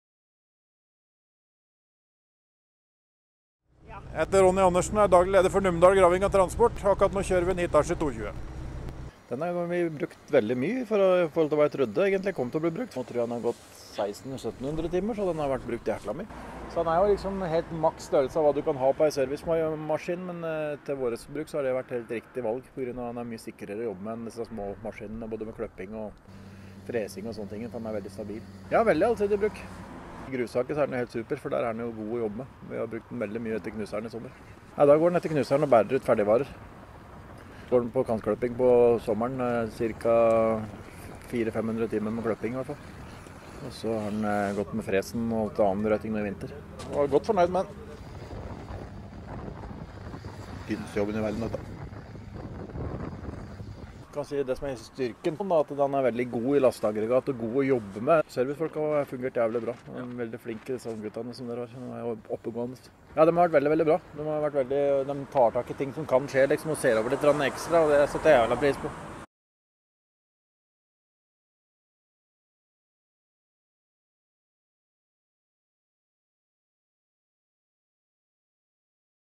ZW220-6 hjullaster hos Numedal Graving og Transport As åpner fjellovergang!
Fineste jobben i verden!
Vi tar turen, sammen maskinselger Øystein Myrhaug hos Nasta, til Imingfjell – fjellovergangen mellom Telemark og Numedal. Gjengen hos Numedal Graving og Transport As er i ferd med å åpne fjellovergangen før påsketurismen setter inn med en Hitachi hjullaster og fjellet lokker derfor ekstra mye. Og forventningene våre innfrir: solen stråler, himmelen er knallblå og en oransje ZW220-6 hjullaster stråler mot oss når vi møter brøytegjengen oppe på fjellet.

Daglig leder Ronny Andersen tar oss godt imot. Han startet i 1996 et enkeltmannsforetak, med en graver og lastebil. Siden den gang har entreprenøren opplevd og vært med på mye. «I 1998 fikk vi en «3 ukers jobb» for Arne Olav Lund – den varte frem til 2013. Vi har også hatt jobber for både Vegvesenet og Mesta. I 2006 så vi det nødvendig å danne et AS. Vi var oppe i hele 40 mann en periode, men det ble helt håpløst. Jeg er ikke skapt for å bare sitte på kontor. Nå er vi mellom 15-20 hele tiden og det er fin størrelse for oss. Alle er poteter her og vi er alle gode til å svinge oss rundt! Dessuten holder vi oss kun i lokalområdet her i Numedal og trives godt med det. Jeg har gode karer som er tilpasningsdyktige, så her stiller vi opp på det meste på kort varsel» smiler sjefen.

Entreprenøren har ansvaret for 9 kilometer av fjellovergangen, samt 2,5 mil av strekningen mot Numedal. Dessuten har de 5 roder for NCC. Fjellovergangen stenges 1. desember og åpnes fredag før påske (eventuelt 1. april hvis det inntreffer først). «Det er synd at Imingfjell er stengt store deler av året. Det er jo helt fantastisk fint her oppe! Dette er den beste jobben i verden dette!» sier Ronny entusiastisk. Hitachi hjullasteren er godt synlig i det hvite terrenget. Den går med en PW-1200 snøfres fra Øveraasen. Hjullasteren ble kjøpt av Numedalsentreprenøren for et år siden. Da hadde den 400 timer på telleren, nå har den rundt 1700. «Hjullasteren har vi brukt veldig mye i forhold til hva jeg trodde vi skulle. Den har vist seg å være veldig allsidig». Foruten vinterbrøyting går lasteren store deler av året i grustak, men også i kantklipp på sommeren: «I grustaket er den helt super! Der går den etter knuser og bærer ut ferdigvarer. Den er god å jobbe med også på kantklipp. Vi har vel cirka 400-500 km per år med kantklipp og det har gått veldig fint».

ZW220-6 hjullaster fra Hitachi har vært en populær modell i Europa. Våren 2021 blir den nye ZW220-7 lansert hos Hitachi. Modellstørrelsen har vært en populær vektklasse også i Norge, og det er ikke så rart at det er denne modellen som er den første ut i rekken av serie 7 lastere. Ronny er bestemt på at hjullasterstørrelsen også er perfekt for dem: «Det er nok maks størrelse til å være en servicemaskin, men til vårt bruk har det vært et riktig valg. Det er mye sikrere. Den er veldig stabil i lasteaggregatet og virkelig god å jobbe med». Entreprenøren har hatt mye Hitachi tidligere, men aldri nytt. Så hvorfor ble det en Hitachi laster i fjor? «Jeg prøvde mange typer og merker, men han (Øystein Myrhaug) var veldig ivrig da. Så da ble det Hitachi» og det virker som om entreprenøren fortsatt ikke har angret på valget. «Det har vært lite feil med den. Men de servicemekanikerne som har vært her har vært jævlig bra. De er virkelig oppegående folk! De er flinke, og tar tak i ting og ser over litt ekstra og det setter jeg virkelig pris på!» før han freser videre i verdens fineste jobb!